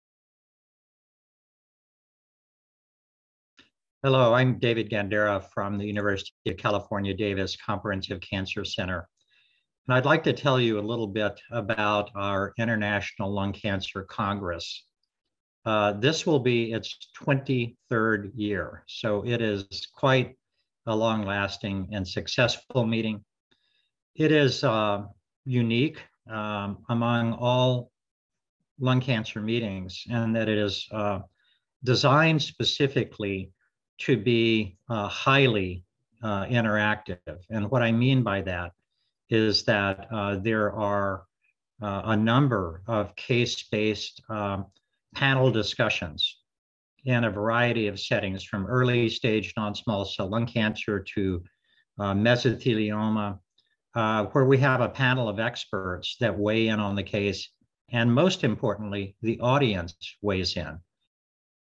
David R. Gandara, MD, invites you to the 23rd Annual International Lung Cancer Congress® 7/28-7/30
David R. Gandara, MD, invites you to the 23rd Annual International Lung Cancer Congress® July 28 - July 30, 2022. Join PER® as we discuss the latest data on targeted agents, immunotherapy, surgery, and radiation oncology for the management of lung cancer.